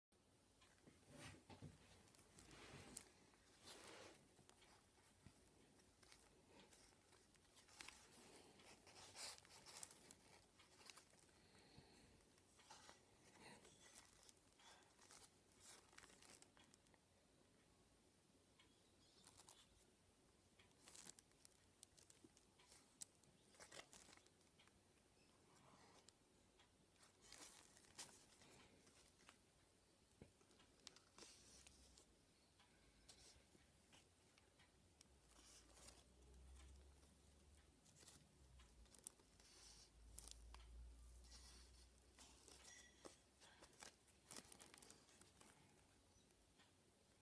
Origami Nazgul Version 8.1 (Jason Ku)
Nazgul from Lord Of The Rings designed by Jason Ku.
Made out of 60x60cm Kraft Paper.